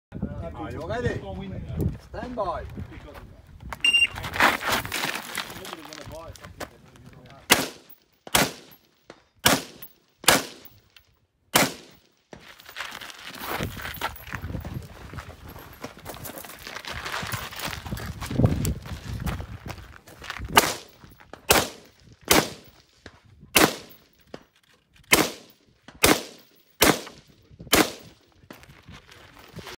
Championnat de France IPSC Rifle 2018 Stage 16
10 Poppers on 190m - 5 small and 5 large ones. 4 on the far left and 4 on the far right, two in the middle. Best way was to shoot 4 far right from the left side of the box, rest from the right. Needed one extra on the left for the last small one and three more on the right for the small one in the middle. this sucker was ducking!